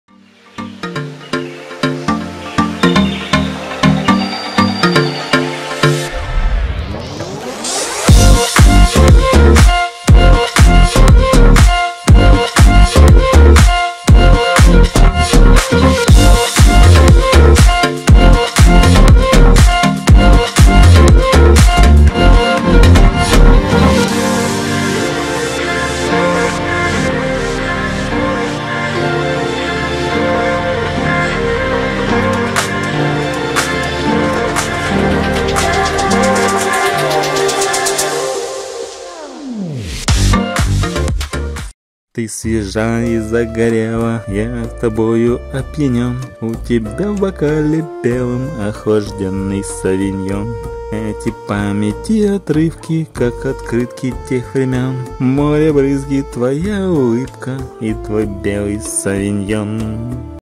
Международный день белого вина «Совиньон Блан» (International Sauvignon Blanc Day)
Международный день белого вина «Совиньон Блан»

Ты свежа и загорела,
Я тобою опьянен,
У тебя в бокале белый,
Охлажденный Совиньон.

Эти памяти отрывки
Как открытки тех времен,
Море, бриз, твоя улыбка,
И твой белый Совиньон.

Художник и поэт Artsky_net!

День белого вина «Совиньон Блан»
Отмечается: ежегодно в первую пятницу мая

Ежегодно в первую пятницу мая все любители белого вина с удовольствием отмечают Международный день белого вина «Совиньон Блан» (International Sauvignon Blanc Day).  Совиньон Блан или просто Совиньон - это один из самых популярных и культивируемых в мире технических (винных) сортов винограда. И известен он, прежде всего, благодаря своим неповторимым вкусовым и ароматическим качествам.

Виноград Совиньон Блан имеет небольшие листья с пузырчато-грубой поверхностью, которые пересечены прожилками, а снизу покрыты пушком. Грозди винограда – маленькие и компактные, а ягоды – небольшие, овальной формы. Когда ягоды созревают, они приобретают красивый золотисто-желтый цвет. Мякоть ягод сочная, ароматная, кожица – толстая. Лоза Совиньон Блан древовидная, что позволяет ей выдерживать умеренные зимние холода.

Для выращивания Совиньон Блан подходят жирные глины или меловые отложения, такие как в регионе средней Луары. В основном, урожайность этого сорта небольшая – 10 гкл/га (в Долине Луара), в Бордо и Бургундии урожайность, как правило, средняя - 45 гкл/га. Однако в последние годы энологи (те, кто обладают наукой о вине) добились существенных результатов в разведении качественных клонов. Сегодня урожайность Совиньон Блан может варьироваться от 50 до 100 гкл/га.

История сорта началась на западе Франции, в регионе Бордо, который считается исторической родиной Совиньона, и Долине Луары, где он больше проявил себя как самостоятельный сорт. В зависимости от климата в букете вина «Совиньон Блан» преобладают либо травяные, либо сладко-тропические нотки. В более прохладных регионах вино получается «классическим», свежим и его не выдерживают в дубовых бочках. В нём чувствуются тона зеленого болгарского перца, маракуйи, бузины, крыжовника.

«Хрестоматийными» можно считать описания Совиньонов из Долины Луары. Они очень свежие, с ароматами крапивы, листьев чёрной смородины, болгарского перца, крыжовника, зелёных яблок и… того, что обычно произносится по-французски – pipi de chat. Последняя из упомянутых нот – очень яркая и характерная черта Совиньона, каким его любят ценители сорта. И пусть вас не смущает, что переводится это, как «кошачье пипи» (именно то, о чём вы подумали!).

Следует упомянуть о том, что вино «Совиньон Блан» лучше пить молодым, так как с возрастом особых результатов оно не демонстрирует. Аналогично «Шардоне», это вино приобрело огромную популярность в 80-х годах прошлого столетия, когда во всем мире в моду стали входить легкие белые вина с освежающей кислотностью.

На сегодняшний день все ценители вина «Совиньон Блан» сходятся в одном — это один из самых ярких сортов винограда, который за небольшой промежуток времени успел стать настоящей мировой звездой. С праздником вас, ценители настоящего вина!

00:00 ►День белого вина!
00:20 ►Международный день белого вина!
00:40 ►International Sauvignon Blanc Day!
00:50 ►Совиньон Блан!